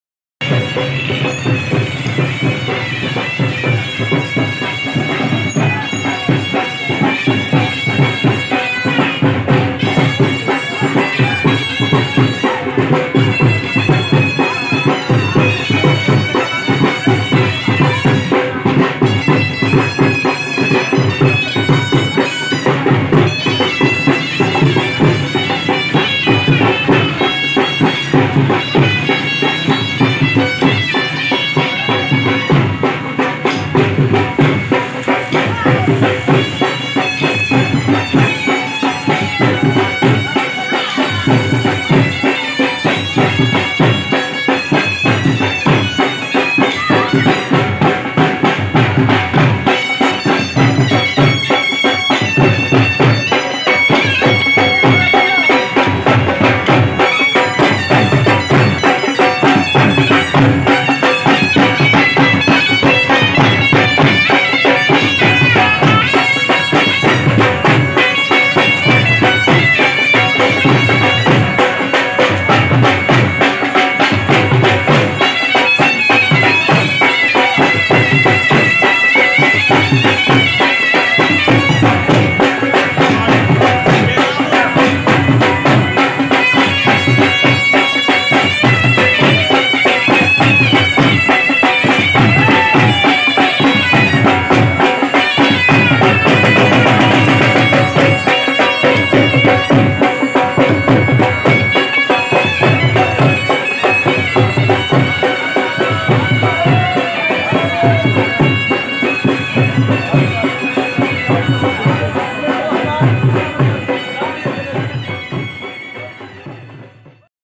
Thathi Jabber to Rawalpindi || Dhamal video || Dali Dhool Beat || Magical Video dhamal 2022
چکڑالی تھاتھی میانہ پوٹھہ اور جبر سے راولپنڈی ڈالی کے مناظر